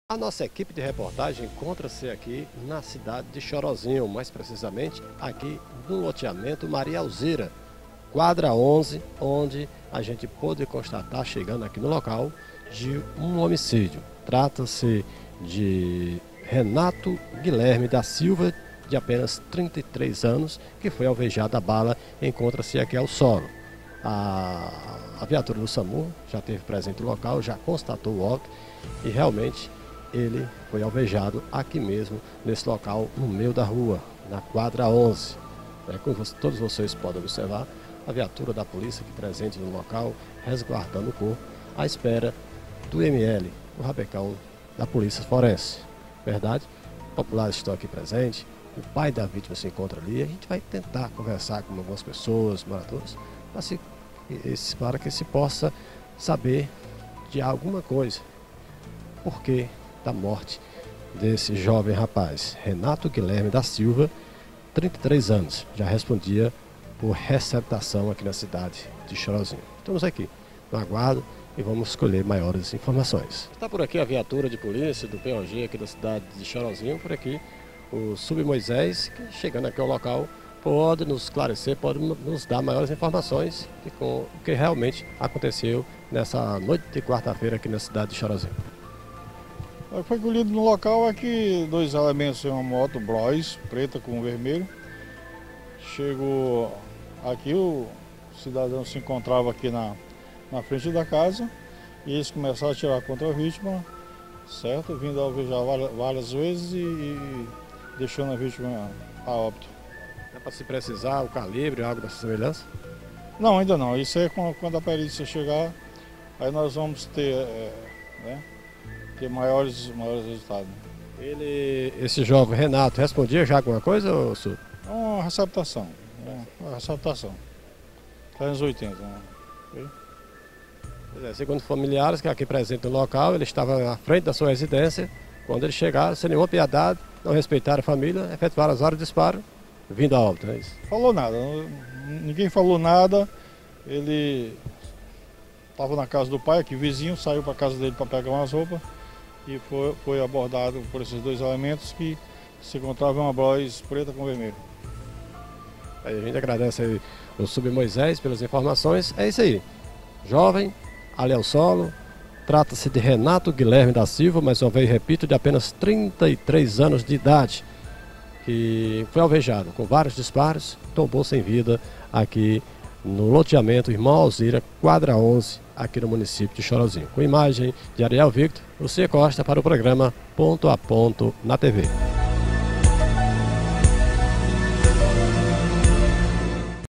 Em Chorozinho-CE/ Polícia registra dois homicidios em menos de 24 horas
A cidade de Chrorozinho que fica na Região Metropolitana de Fortaleza, vem sofrendo com a violência que tem tomado conta da cidade, Quarta feira dia 22 de junho mais um homicídio foi registrado. Esse foi o segundo em menos de 24 horas.
  Maiores informações na reportagem de Lucier Costa. Imagens: Ariel Vitor. Edição de imagens: Iago Silva.